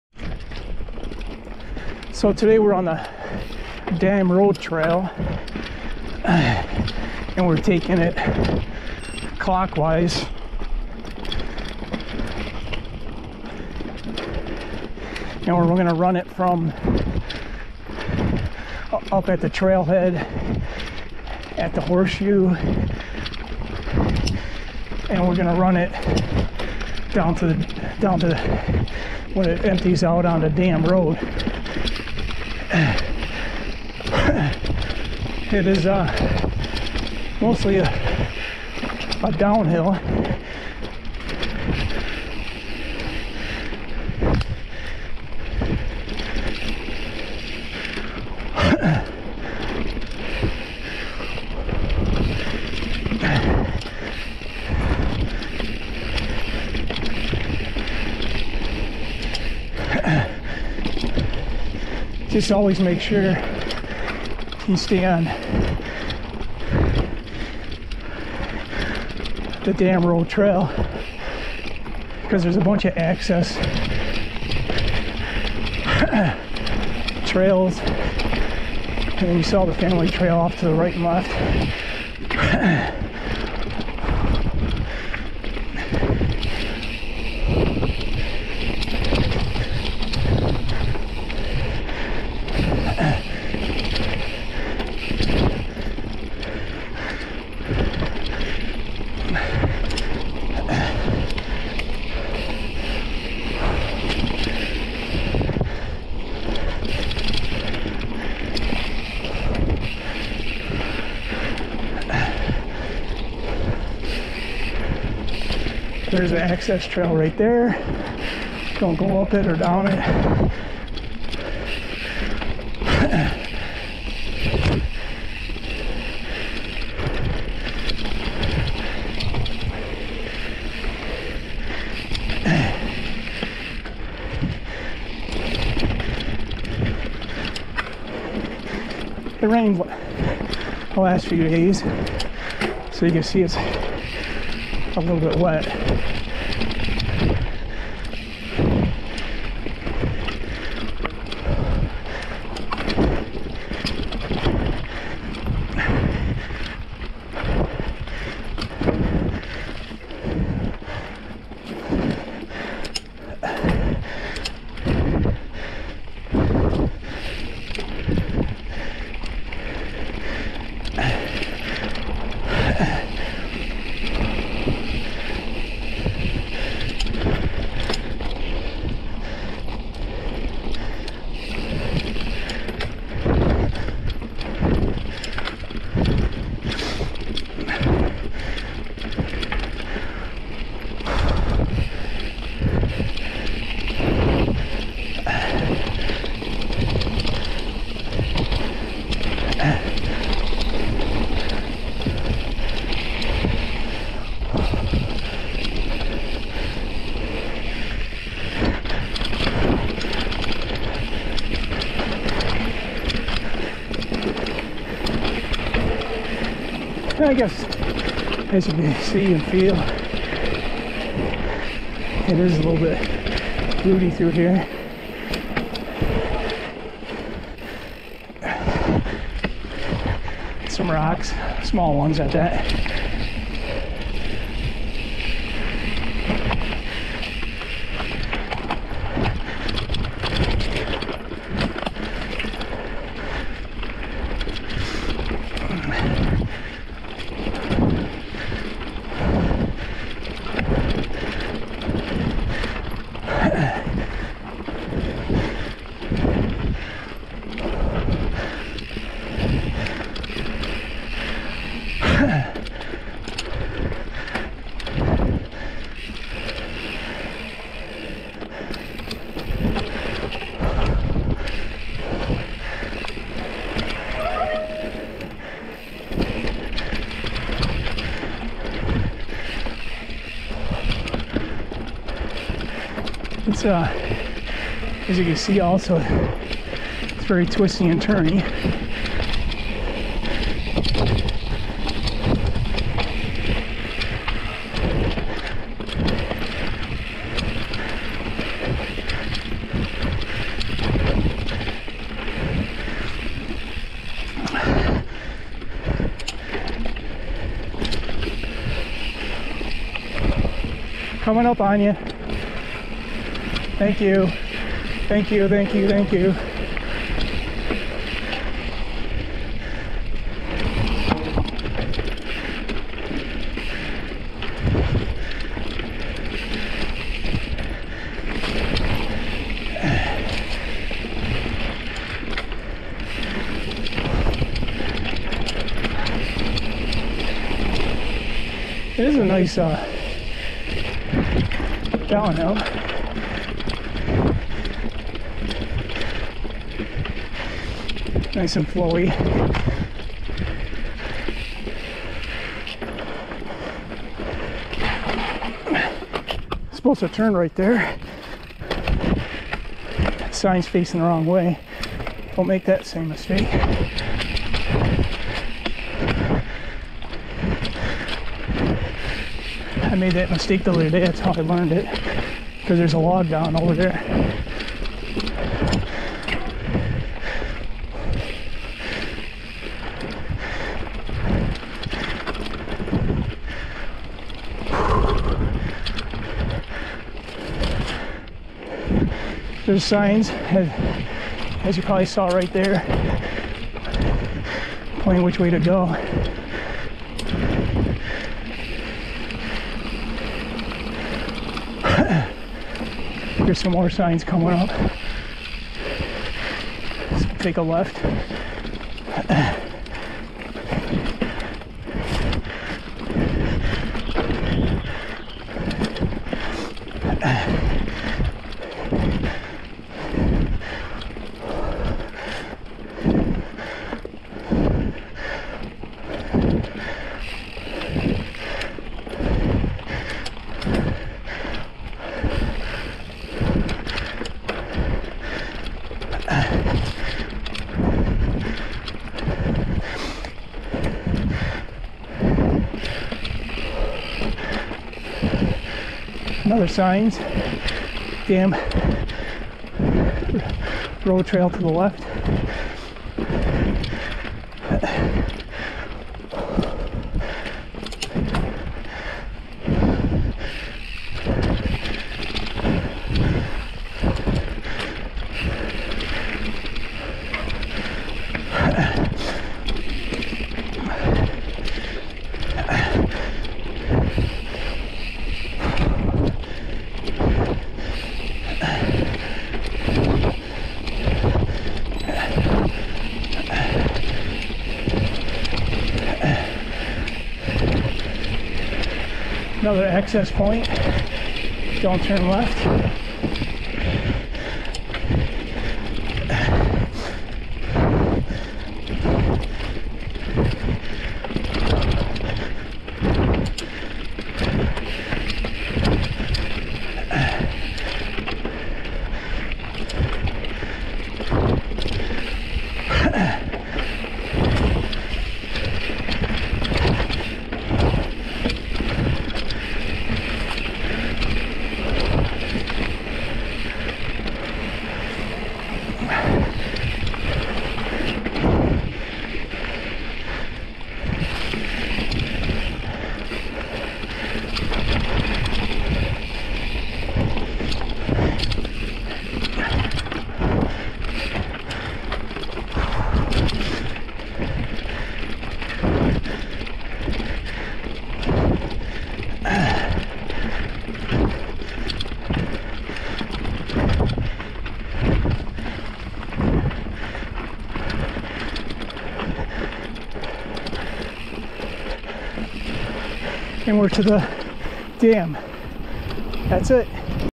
Clemson Experimental Forest- Dam Rd Trail from Horseshoe Trailhead to Dam Rd
Today, I am riding the Dam Rd Trail from the Horseshoe Trailhead to Dam Rd.  The Dam Rd Trail is mostly a fun downhill all the way to Dam Rd.  You will have a couple minor climbs along the way.  The Dam Rd Trail has some technical spots along the way with rocks, roots and switchbacks. You can take the Dam Rd Trail down to the dam to ride the Issaqueena Trail to loop back to the Horseshoe Trailhead.  So let'er rip on the Dam Rd Trail!  (* Video is Narrated.)